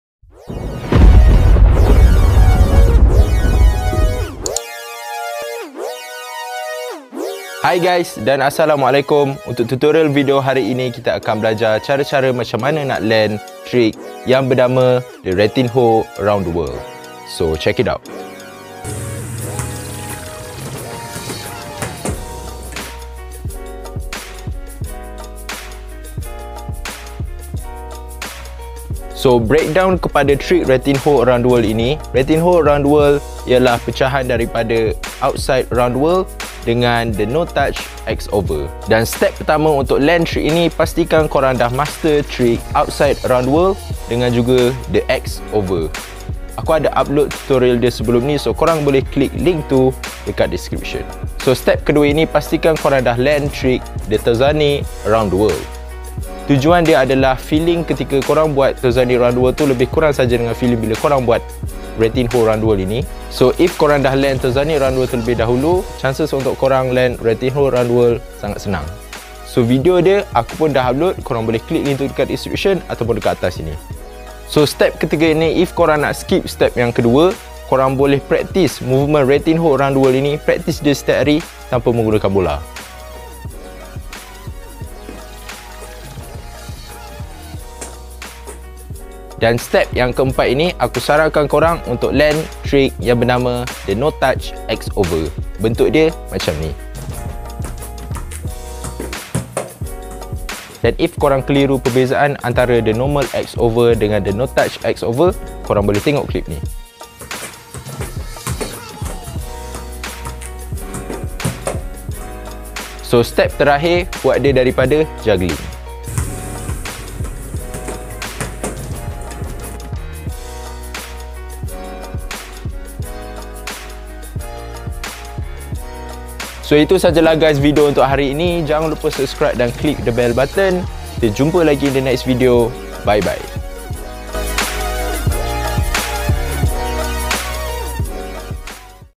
⚽ RATW TUTORIAL || Malaysia Freestyle Football Tutorial
Ratinho Around The World (Ratinho ATW) merupakan salah satu unique atau creative trick dalam football freestyle.

Nak kata trick ini rare, tak juga sebab once korang dah land tatw, trick ini confirm senang gila nak land.

Cuma trick ini rare sedikit la untuk kita lihat freestyler2 famous yang guna trick ini dalam combo. Salah sorang yang aku selalu nampak yg pakai trick ni dlm combo is PWG freestyle.

Overall, it's not that hard, you guys will land it with ease for sure. All the best! :D